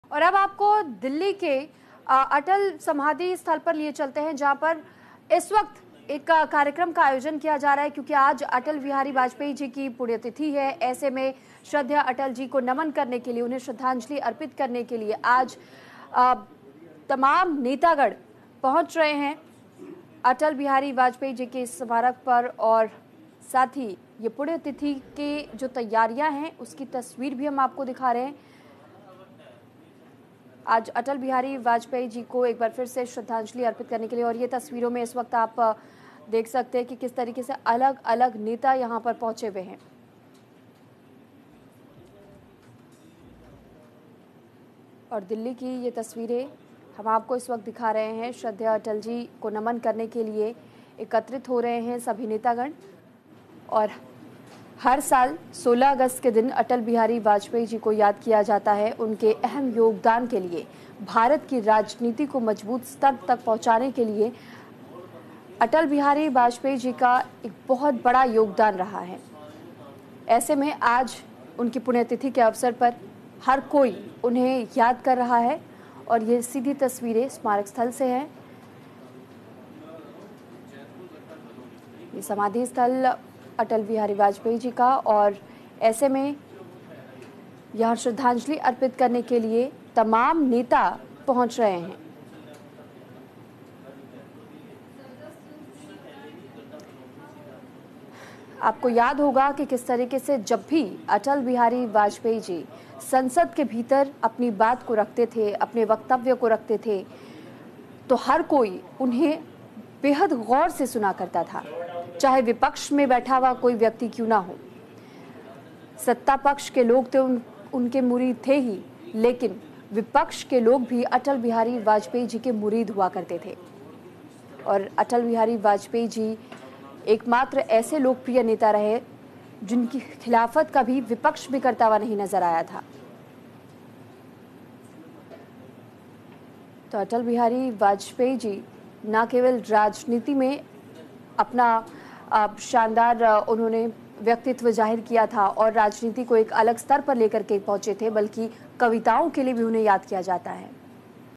Atal Bihari Vajpayee Death Anniversary: पूर्व प्रधानमंत्री अटल बिहारी वाजपेयी की आज पुण्यतिथि | BJP
पूर्व प्रधानमंत्री अटल बिहारी वाजपेयी की पुण्यतिथि आज, देशभर में BJP आयोजित करेगी कार्यक्रम

Today is the death anniversary of former Prime Minister Atal Bihari Vajpayee, BJP will organize programs across the country.